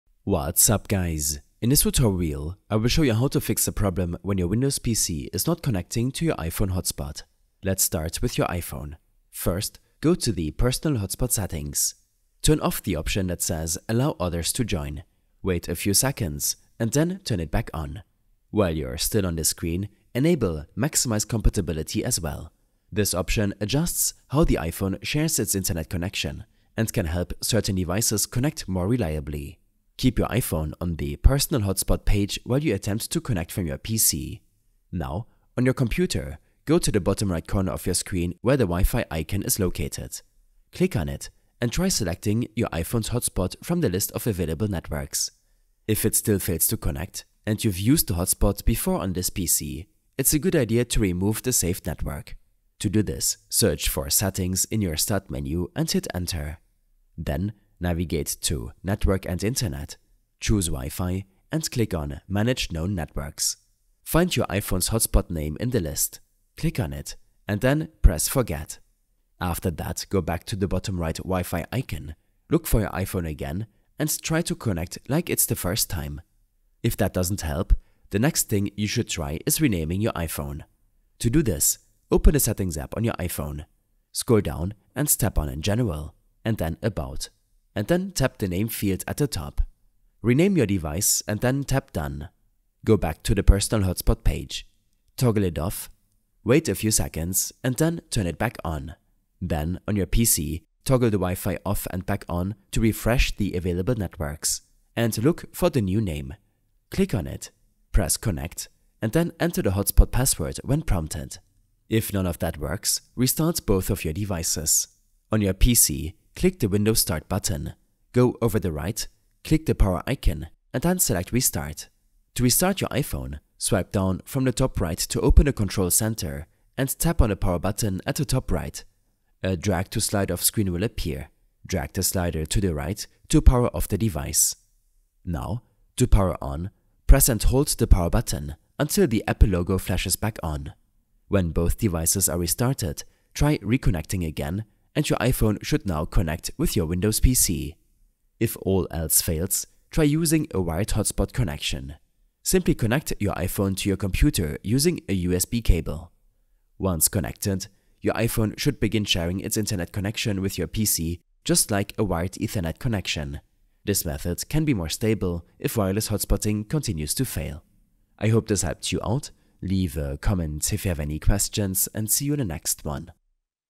How To Fix Windows PC Not Connecting to iPhone Hotspot - Tutorial
A short tutorial on fixing the problem when your Windows PC is not connecting to your iPhone hotspot.